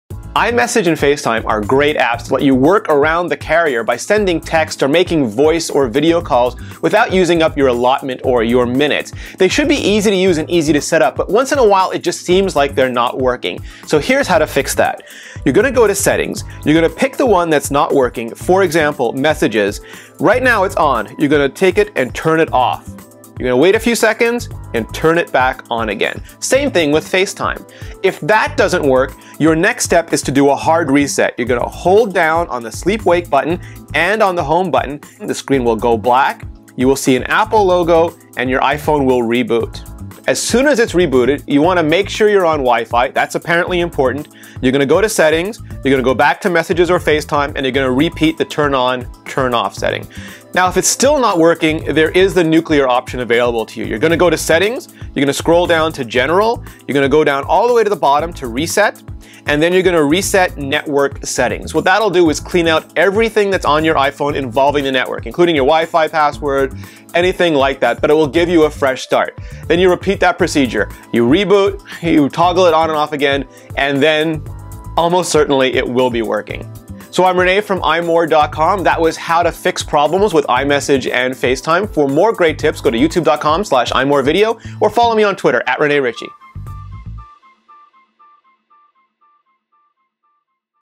Having trouble with iMessage or FaceTime? Here's the fix!
iMessage and FaceTime are wonderful, empowering, enabling technologies that... can frustrate the crap out of you at times. One of those times is when you update or restore your iPhone, iPod touch, or iPad to a new operating system - like millions of people just did with iOS 7 - and it stops working. If you're having trouble with iMessage or FaceTime, if they won't activate or simply won't work, here are some things you can try: